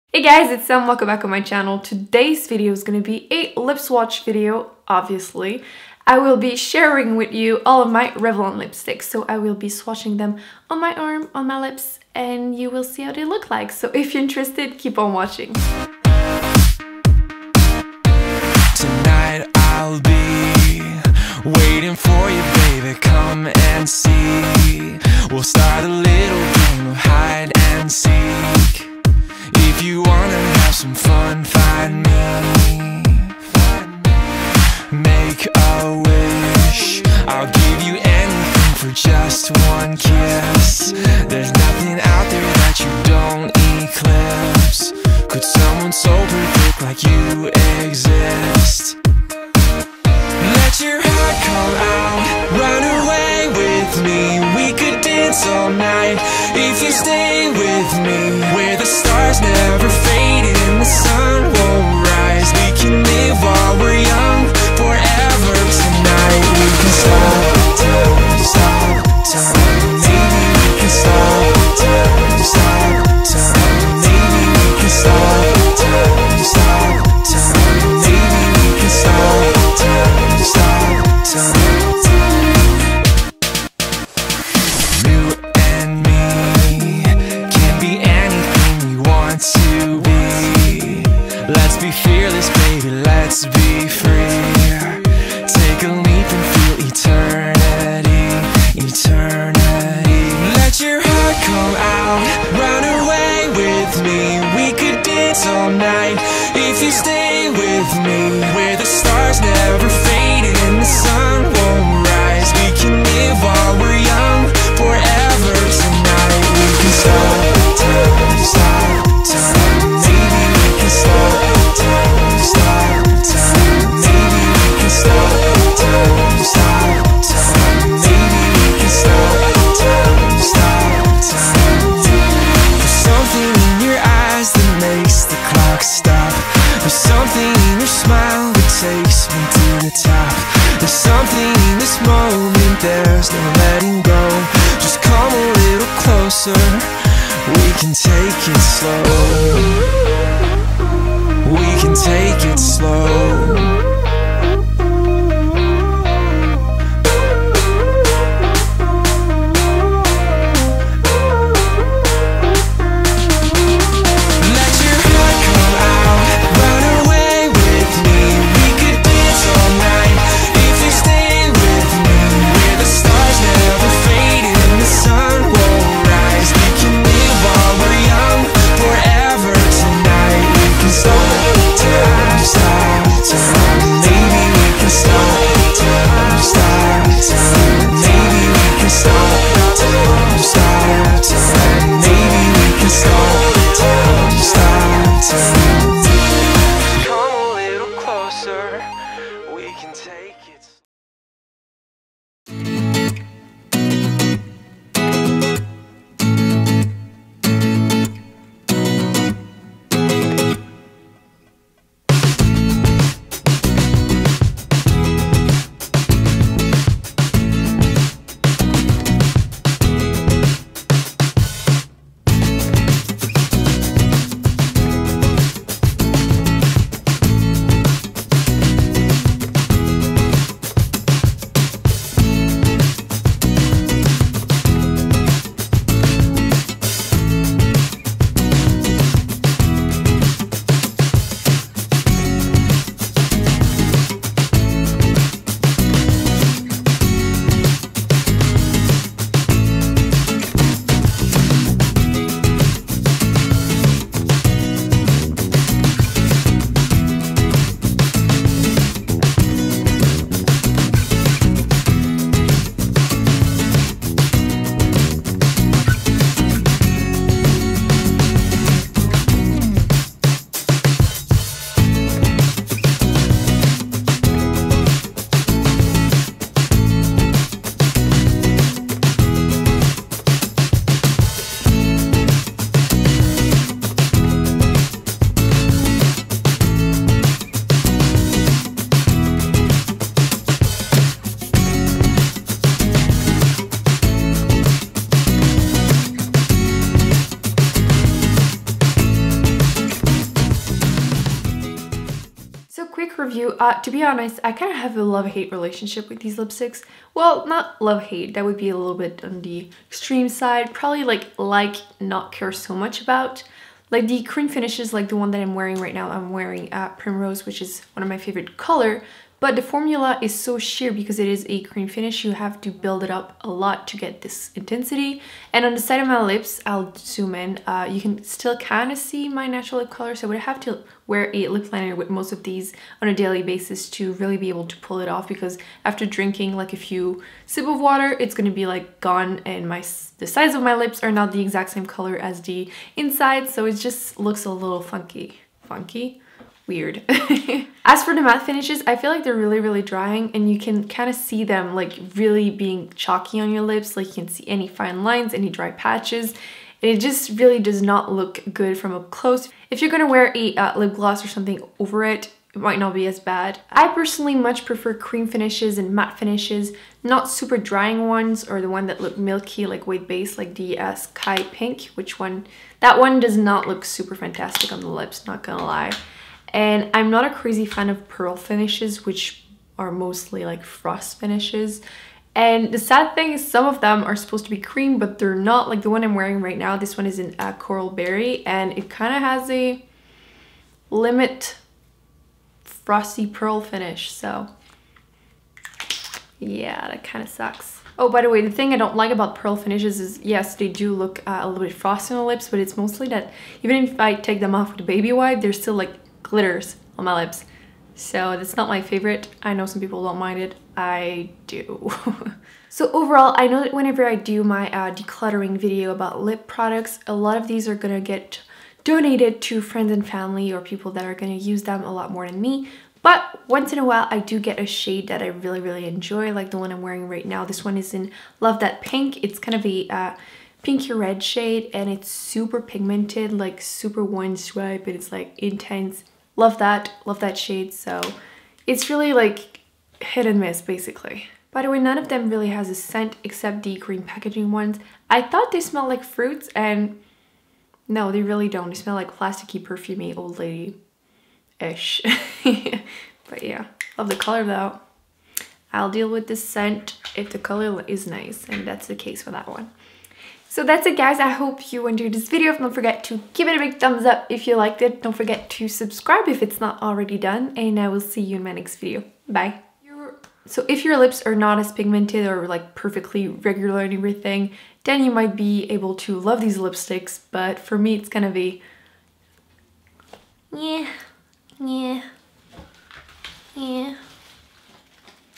Revlon Lipstick : Super Lustrous and Moon Drops + Lip Swatches
Today I'm swathing the Revlon Super Lustrous and Moon Drops lipsticks I own! I have a complicated relationship with these lipsticks and I was trying to find the best way to explain it.
I love the colors but the formula is "hit and miss" for me.
I love cream and matte finishes but only if they’re pigmented enough that they look good on my pigmented lips!
Some of them look gorgeous on and I love, some of them don’t look right alone!
For example, Sky Pink is really white base and doesn't look right on my lips but Smoked Peach, which is also a matte, looks fine!
Same for Coralberry, it doesn't look perfect on my lips plus it almost looks like a frost but I love Love That Pink!
Em :)

Lova That Pink (Moon Drops) - Crème
Persian Melon (Moon Drops) - Crème